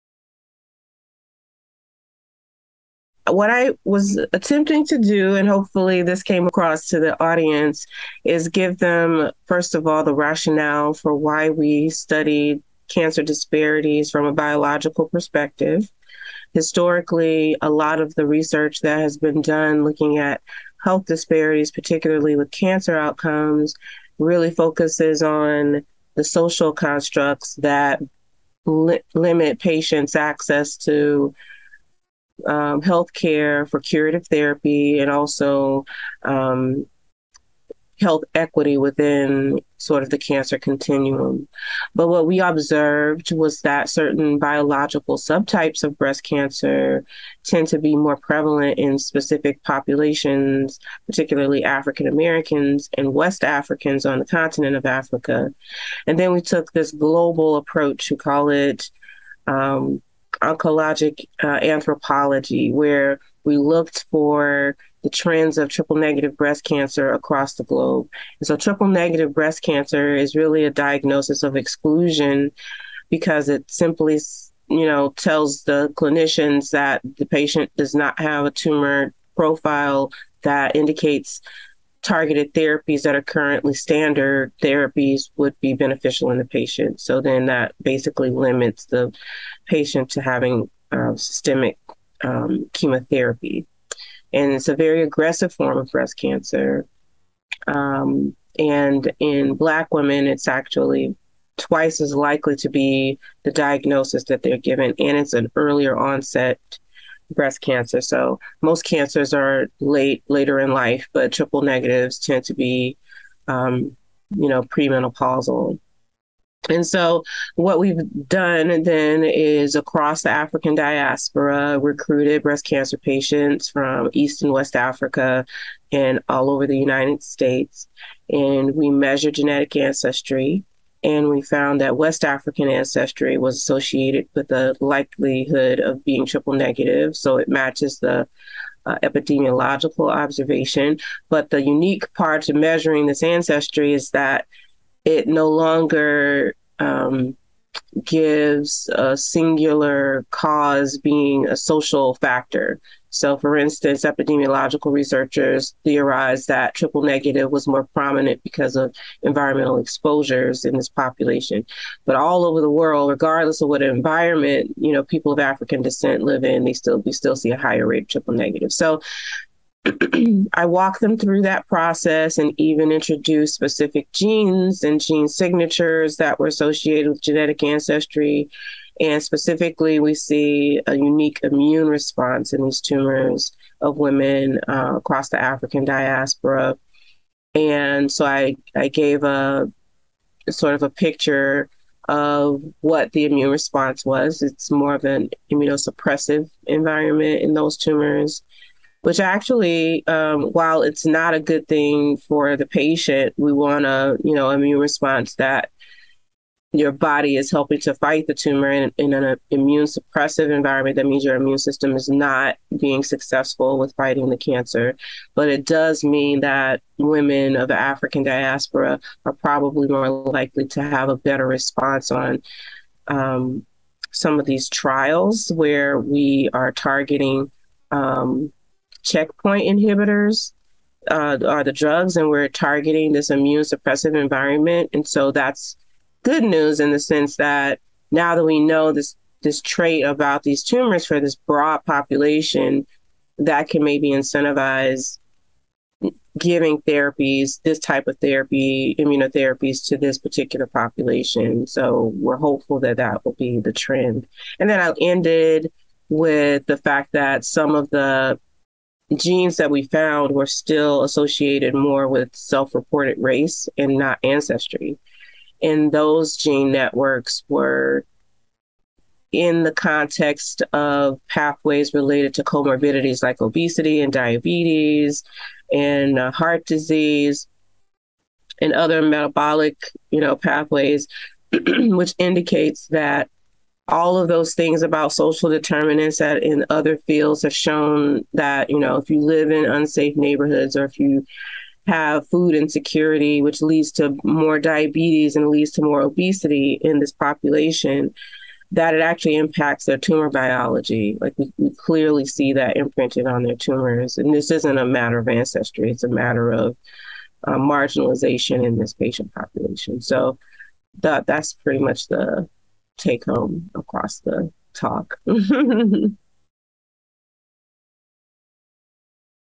Breast cancer in African American women
Melissa Boneta Davis, Phd, Weill Medical College, New York City, NY, discusses an exploration of breast cancer in African woman, focusing on cancer disparities from a biological perspective, health disparities in regard to cancer outcomes and the social constructs that limit patients access to healthcare for curative therapy. Finding on an investigation into the trends of triple negative breast cancer (TNBC) across the globe showed a TNBC diagnosis is twice as likely in Black woman. Patients with breast cancer were recruited from East and West Africa and the USA to assess their genetic ancestry, showing a unique immune response in the tumors of woman across the African diaspora. This interview took place at the Molecular Analysis for Precision oncology (MAP) Congress 2022 in Amsterdam, Netherlands.